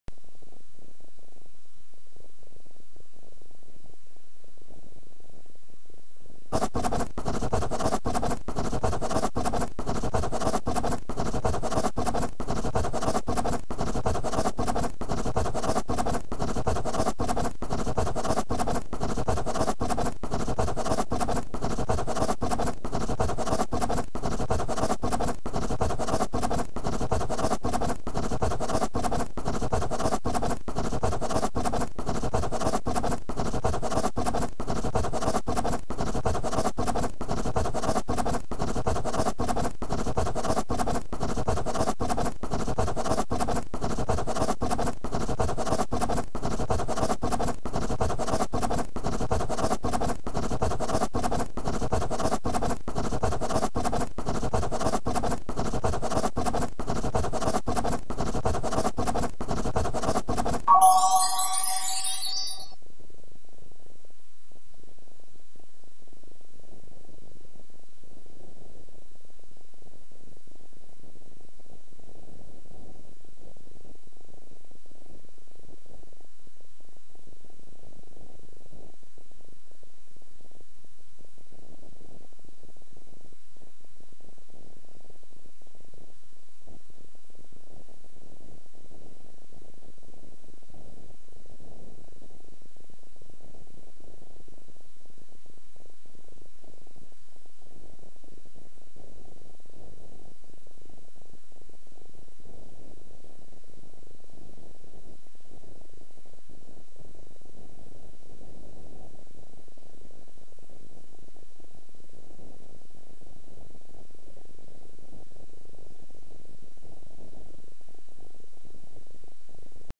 Hour of Code - Artist - Level 10 - Turkey FLAG
Turkey Flag
Now go ahead and make whatever picture you'd like! You've even got an empty function that you can make do whatever you want!